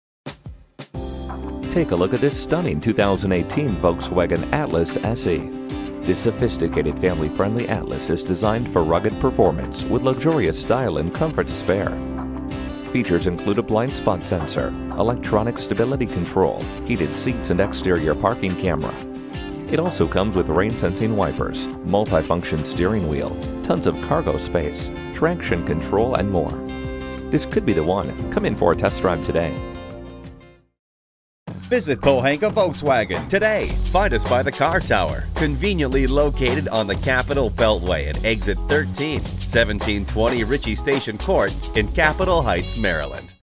New 2018 Volkswagen Atlas Capitol Heights, MD VJC516044 - SOLD
Year: 2018
Make: Volkswagen
Model: Atlas
Trim: SE
Engine: 3.6L V6
Transmission: 8-Speed Automatic with Tiptronic
Color: Black
Interior: Titan Black Lea
Stock #: VJC516044
VIN: 1V2LR2CAXJC516044

Address:
1720 Ritchie Station Court